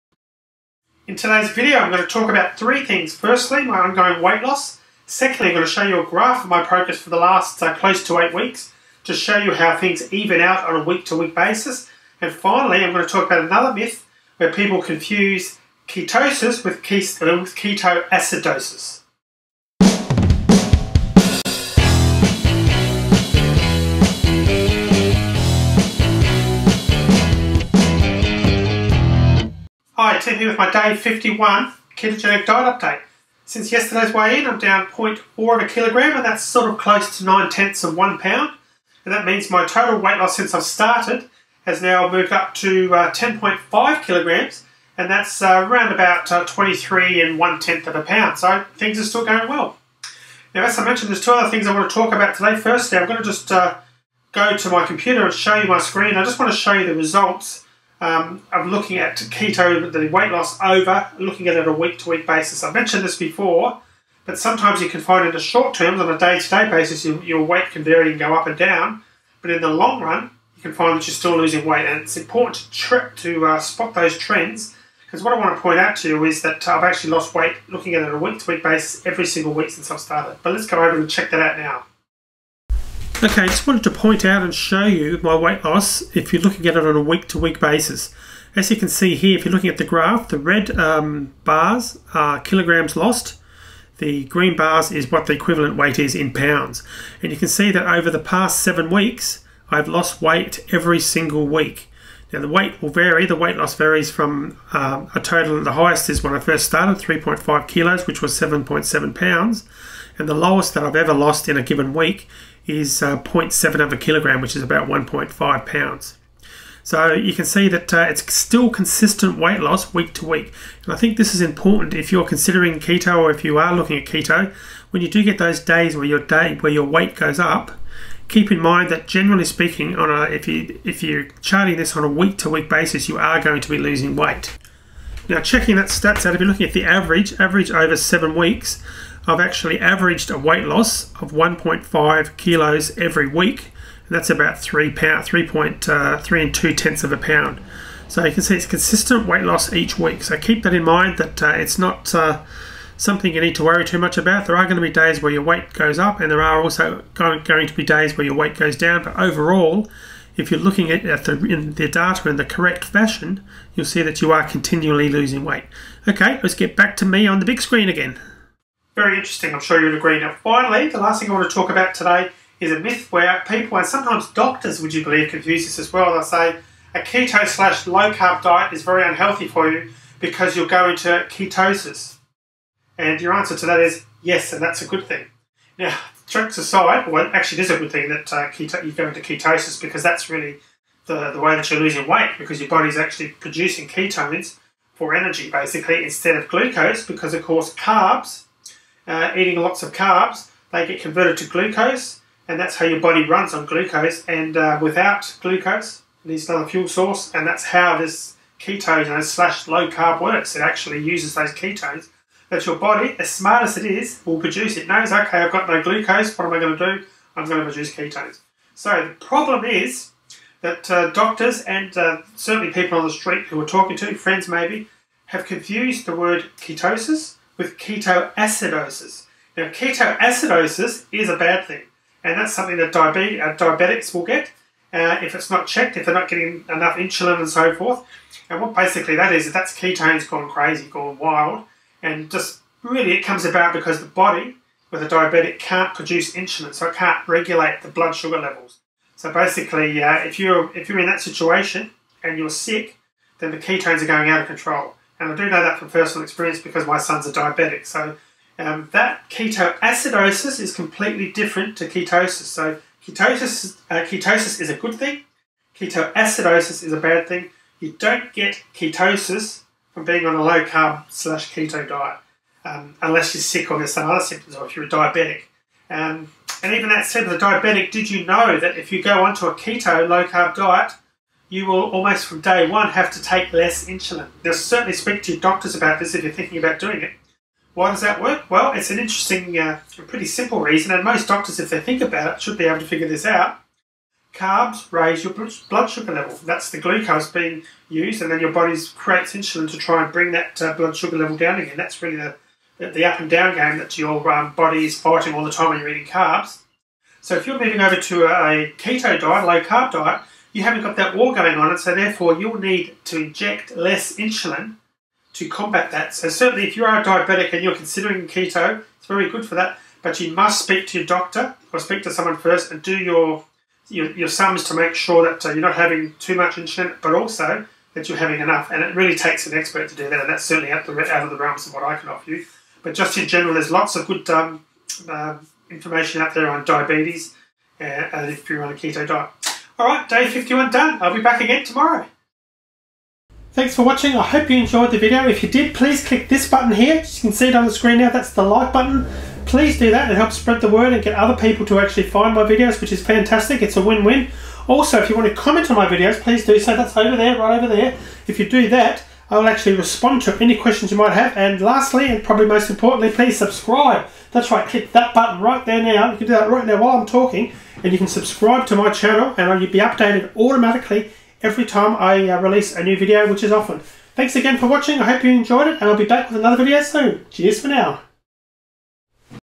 Day 51 - Ketosis And KetoAcidOsis - Yes They Are Different
Hi Tim here with my day 51 ketogenic diet update.

Weight loss charting 101 and myth busting time today.

I am down .4Kg (8/10's of one pound) today.  That brings my total weight loss to 10.5 Kg (23 1/10 of a pound) since I started.

I wanted to discuss how your weight loss evens out over a week period whereas its often possible to gain weight in the short term with Keto.

I show my results on a week to week basis via a chart today to explain this.  Good news if you are worried about weight gain or plateau!

The myth to busy today is where people (even Doctors) come up and say "oh low carb diet is not healthy because you go into Ketosis)".

I explain why this is NOT a problem and is in fact healthy in todays video and talk about ketoacidosis, which is a completely different thing and is where people get confused.

Take a look and let me know what you think.